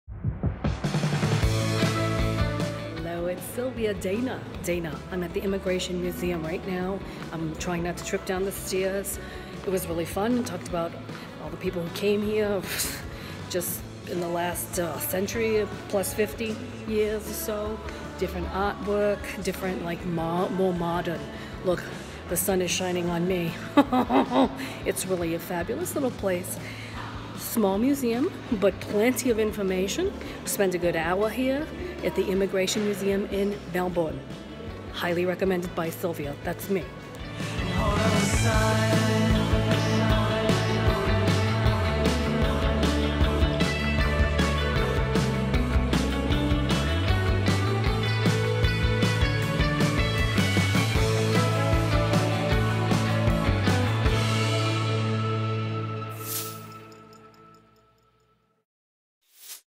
Immigration Museum Melbourne
A simple history that stand on the same place immigration has been grated or not for 150 plus years. Small and interesting.